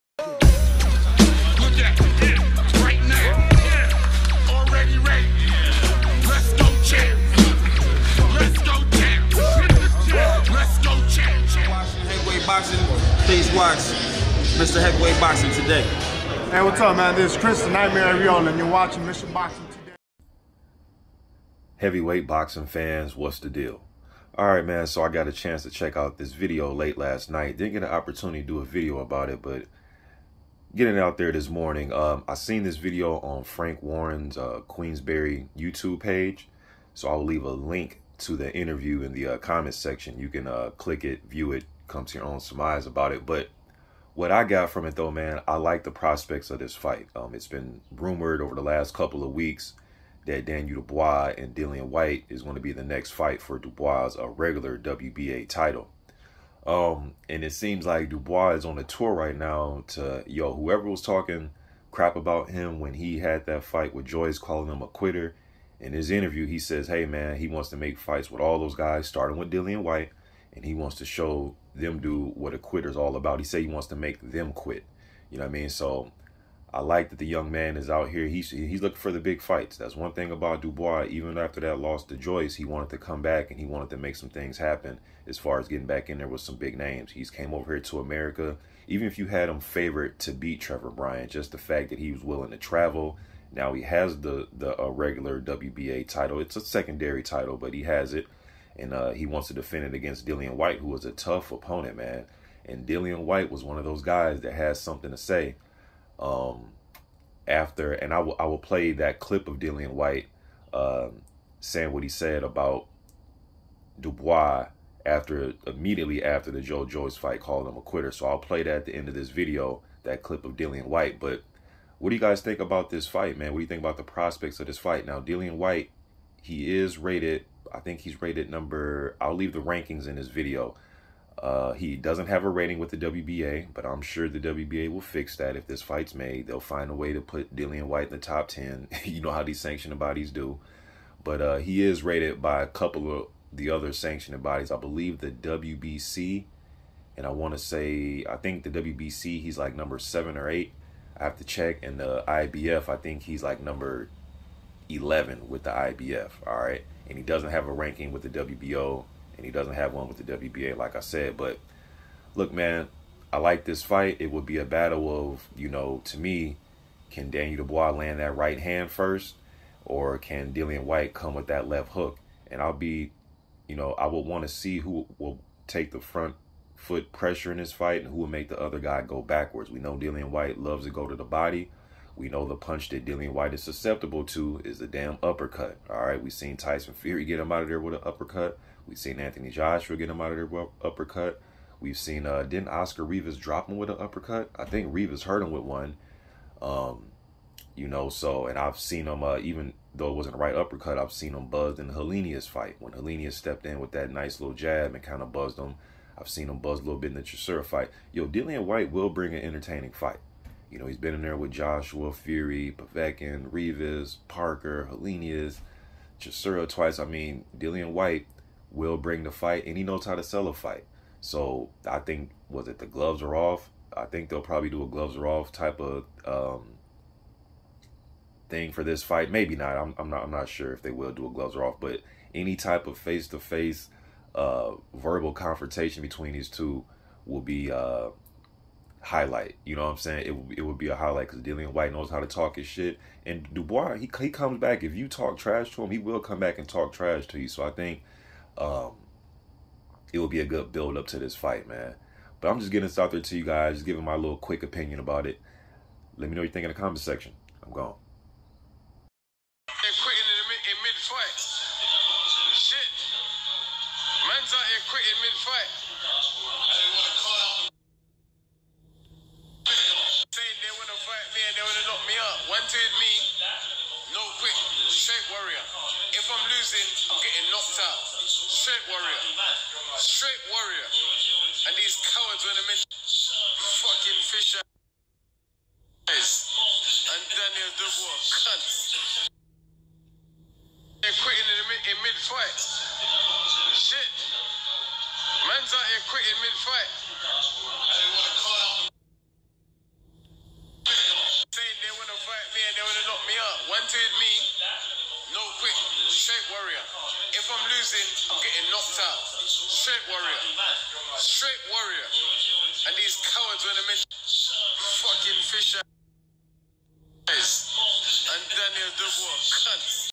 Daniel Dubois Wants To Make Dillian Whyte Quit!!!🤔My Thoughts 🥊🥊
My Thoughts on Daniel Dubois wanting to fight Dillian Whyte🥊🥊